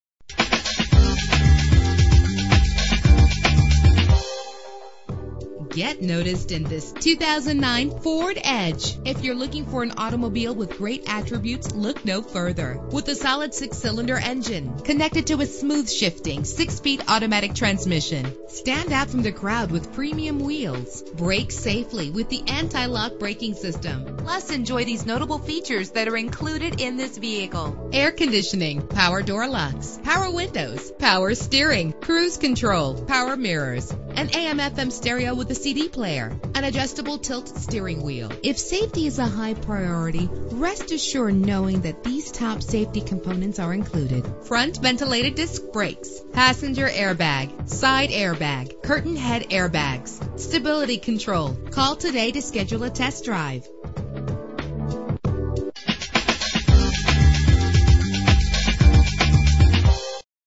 2009 Ford Edge - Roseville CA
Year: 2009
Make: Ford
Model: Edge
Trim: SEL
Engine: 3.5 liter 6 cylinder 24 valve
Transmission: 6-Speed Automatic
Color: White
Mileage: 33543
Address:
650 Auto Mall Dr